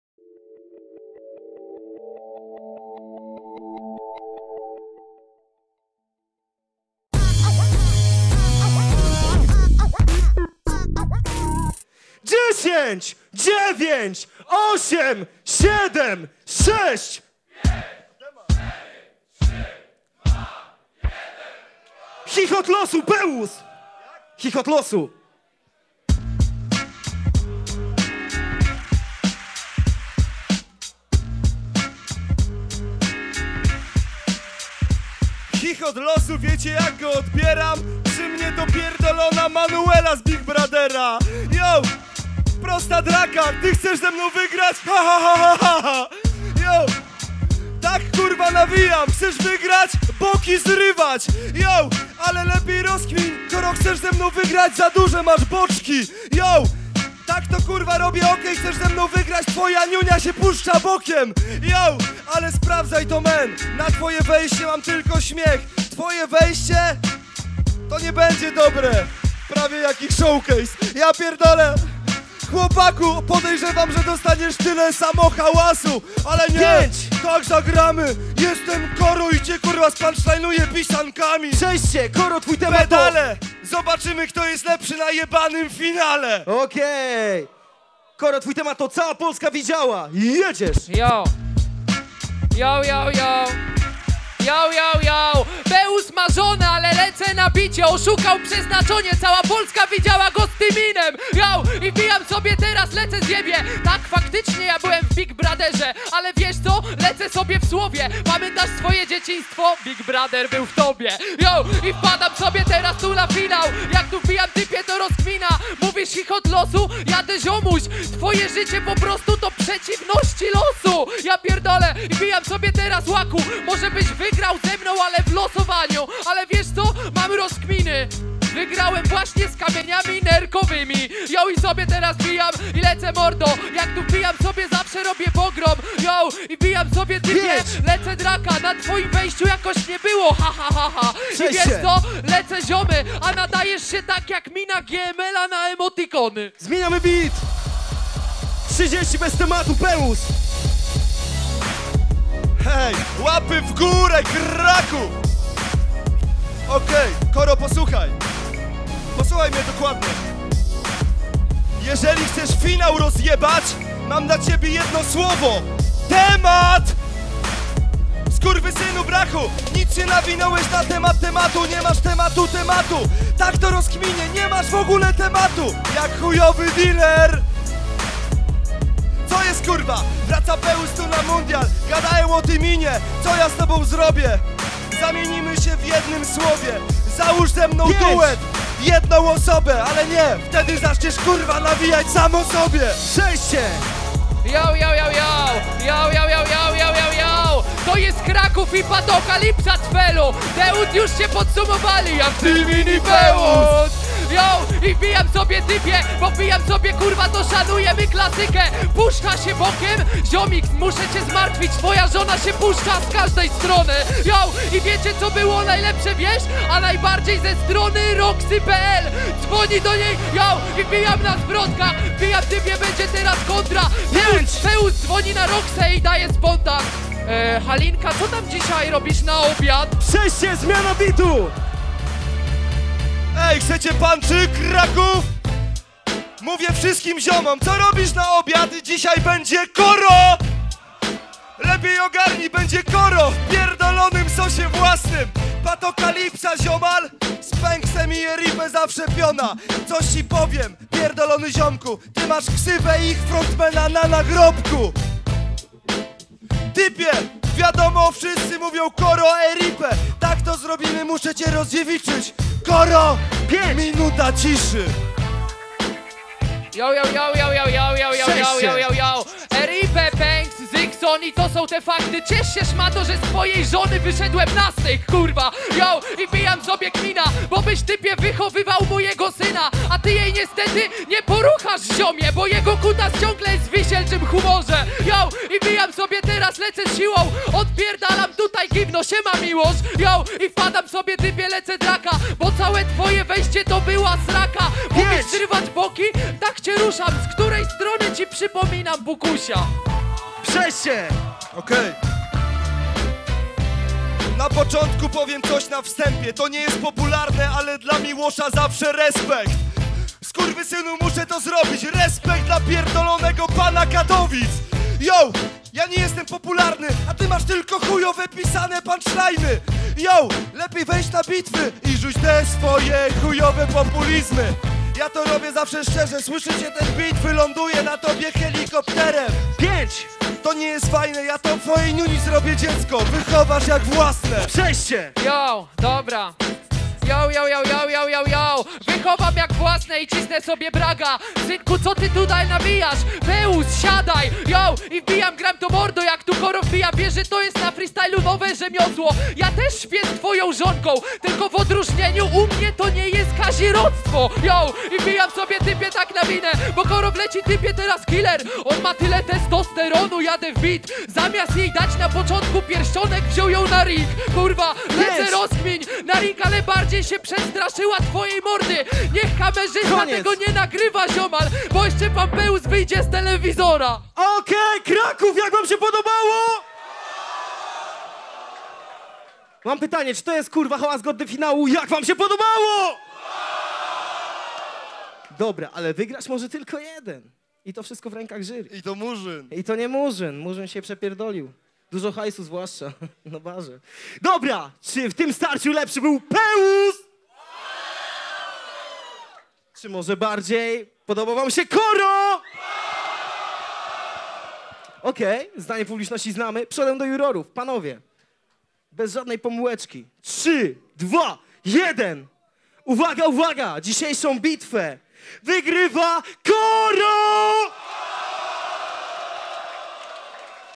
Koro 🆚 Peus 🎤 WBW 2018 Finał Południe (FINAŁ) Freestyle Battle
freestyle: PEUS vs KORO
prowadzenie: MURZYN
dj: AVENS

WBW 2018 Finał Południe - Freestyle Battle,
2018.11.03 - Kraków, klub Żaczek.